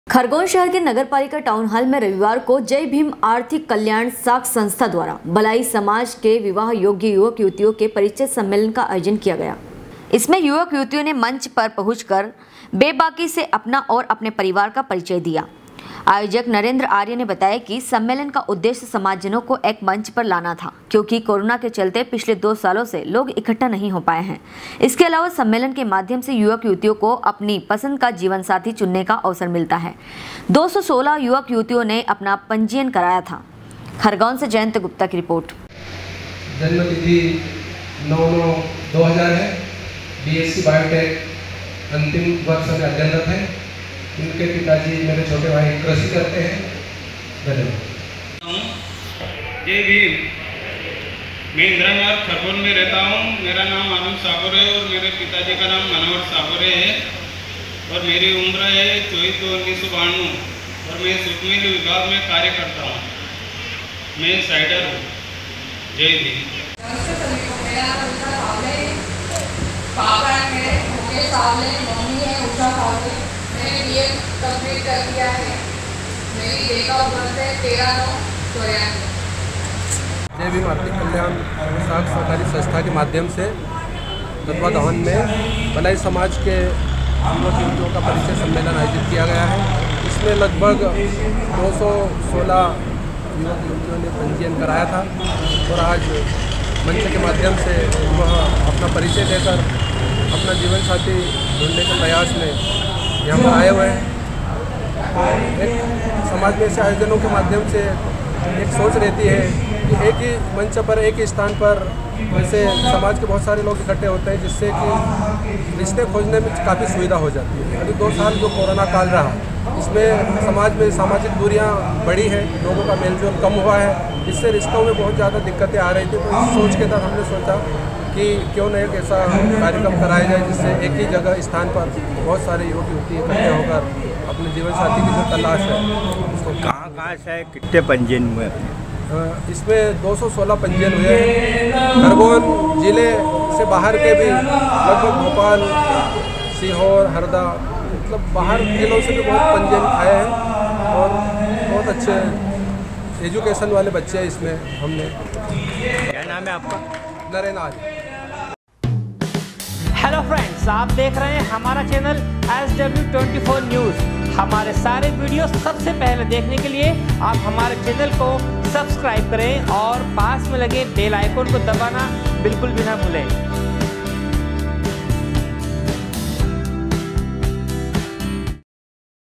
SW 24 News Madhyapradesh
खरगोन शहर के नगर पालिका टाउन हॉल में रविवार को जय भीम आर्थिक कल्याण साख संस्था द्वारा बलाई समाज के विवाह योग्य युवक युवतियों के परिचय सम्मेलन का आयोजन किया गया। खरगोन से जयन्त गुप्ता की रिर्पोट।